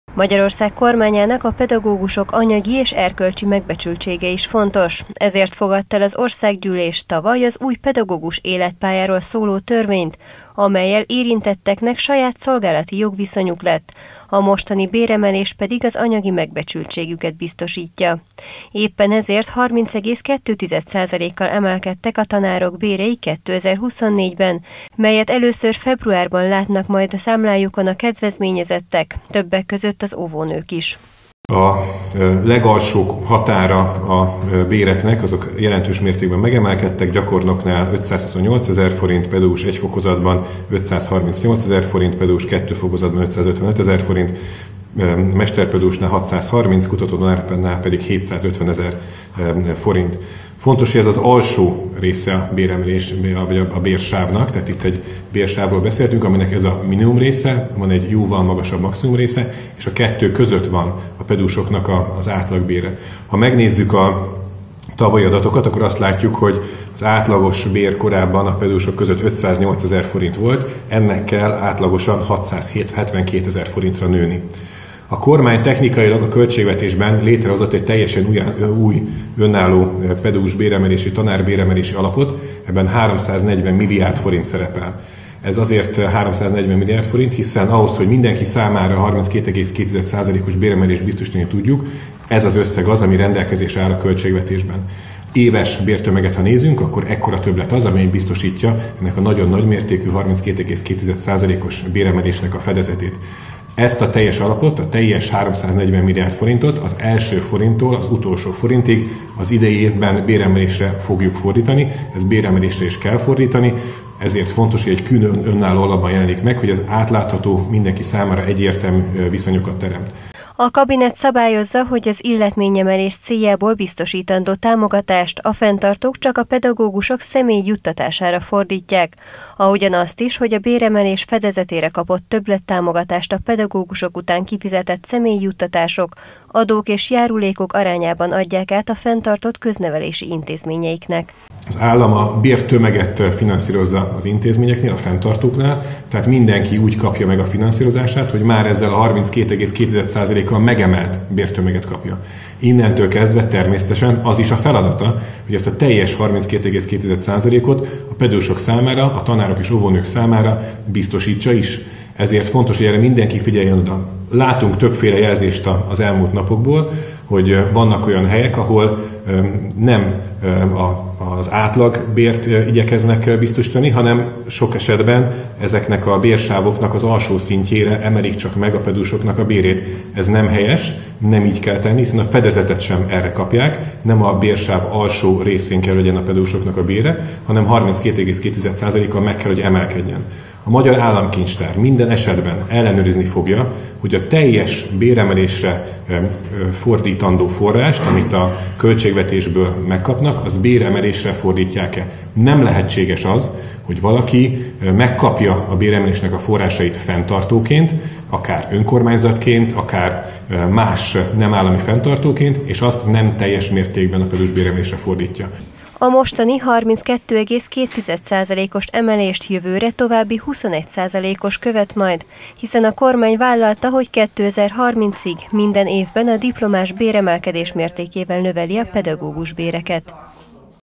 Krónika: Bejelentések a pedagógusok béremeléséről (2024.02.02.)
A következő években minden tanárnak és óvónőnek emelkedni fog a fizetése. 2024. január elsejétől átlagosan 32,2%-kal nő az érintettek havi bére, mivel a kormány vállalta, hogy 2030-ig minden évben a diplomás béremelkedés mértékével emeli a pedagógusbéreket - emelte ki  Rétvári Bence, a Belügyminisztérium parlamenti államtitkára a Magyarország Kormányának hivatalos közösségi oldalán közzétett videóban.